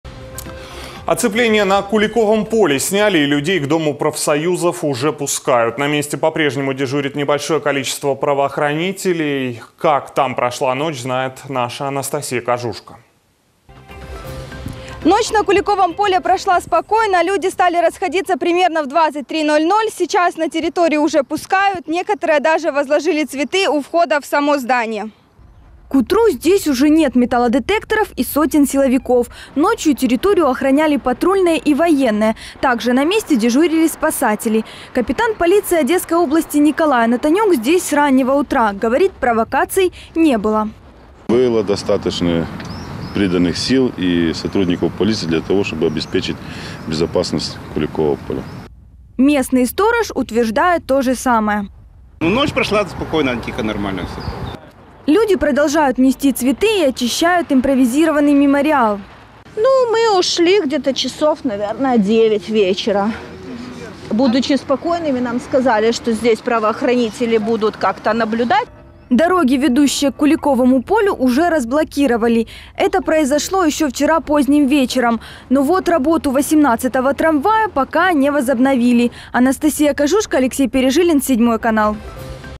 Людей к Дому профсоюзов уже пропускают
Оцепление на Куликовом поле сняли и людей к Дому профсоюзов уже пропускают. На месте по-прежнему дежурит небольшое количество правоохранителей.
К утру здесь уже нет металлодетекторов и сотен силовиков. Ночью территорию охраняли патрульные и военные, также на месте дежурили спасатели. Капитан полиции Николай Анатонюк здесь с раннего утра. По его словам, провокаций не было: «Было достаточно людей, приданных сил и сотрудников полиции для того, чтобы обеспечить безопасность Куликова поля». Местный сторож утверждает то же самое. Люди продолжают нести цветы. Дороги, ведущие к Куликовому полю, уже разблокировали. Это произошло еще вчера поздним вечером, но работу трамвая № 18 пока не возобновили.